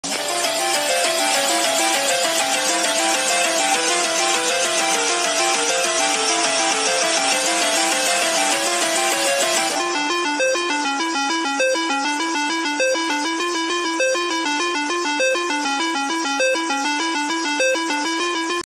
end is near
a short meme achieved now you can see more coming later on for improvement later on, madrigal the cute idol robot is the character in this meme of today.

character: project: madrigal (or just madrigal)

credits:

song by kisker

madrigal by me

animation by me (again)